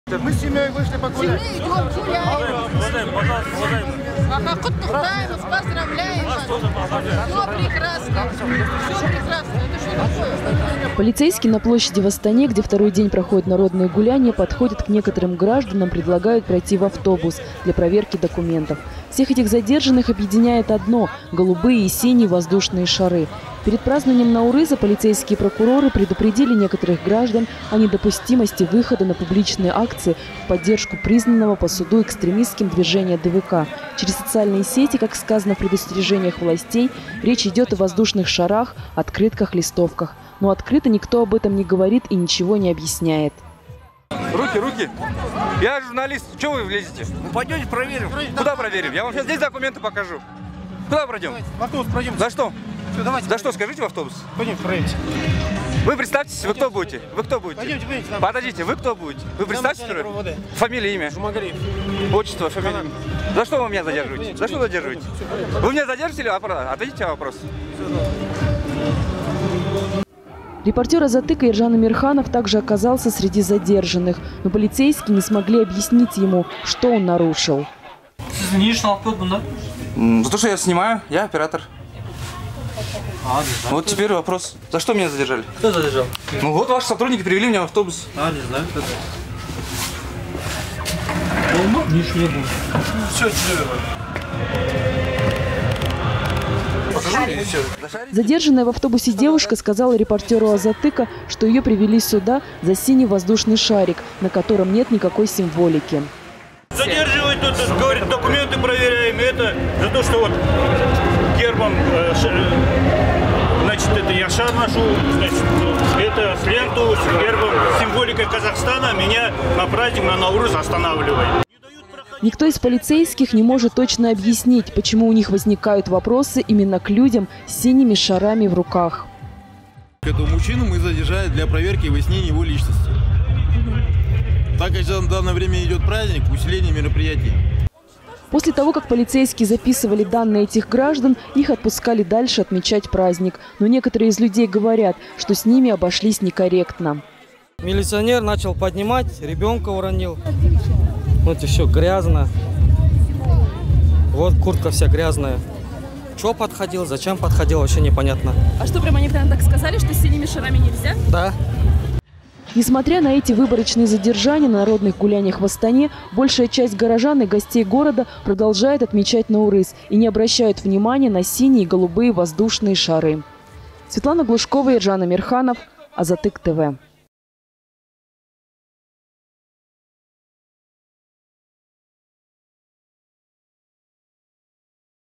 Синие шарики на Наурыз
В Астане на площади у акимата города полицейские задержали несколько жителей города, у которых в руках были синие и голубые воздушные шары. Также на короткое время был задержал и отведен в полицейский автобус репортер Азаттыка Ержан Амирханов.